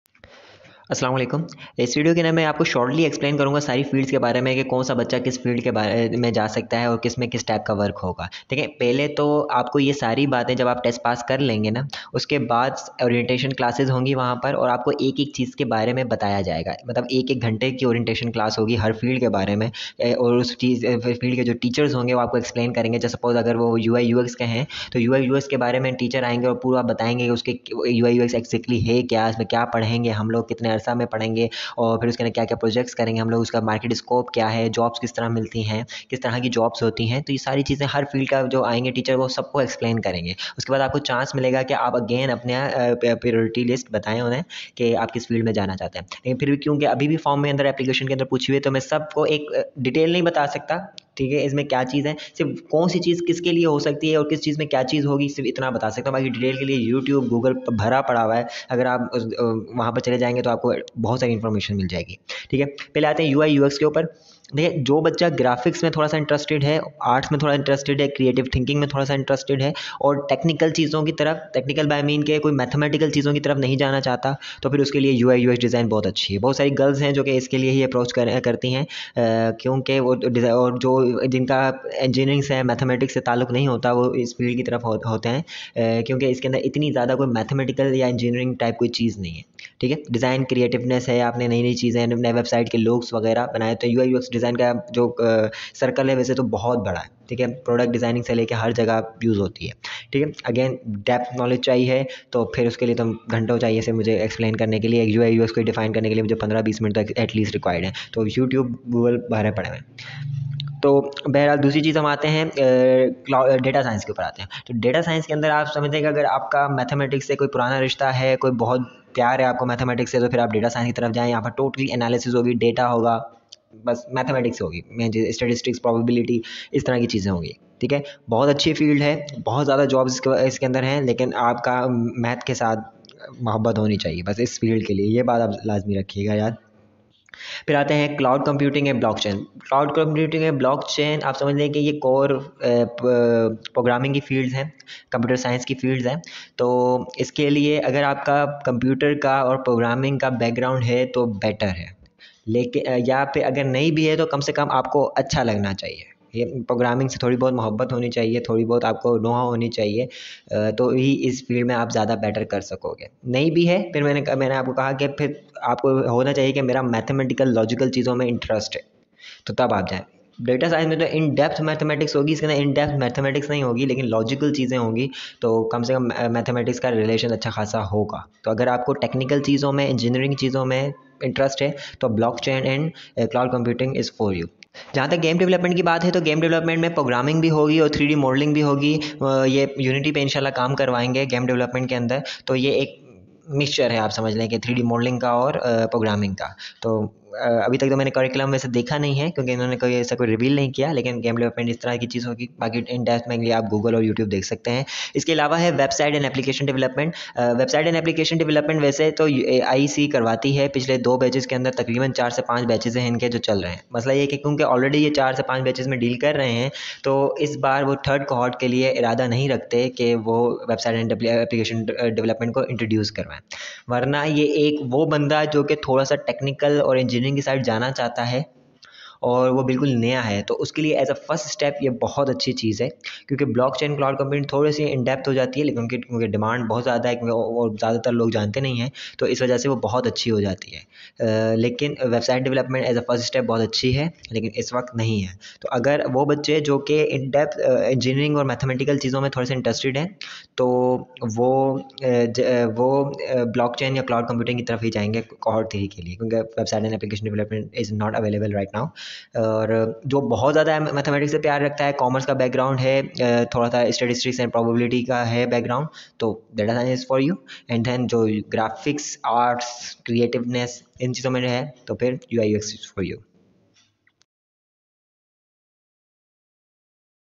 Which field should I choose?? UI/UX? Blockchain? CloudComputing? DataScience? Game development?
UI/ux ??
data science ??
blockchain ??
web development ??
game development ?
cloud computing ??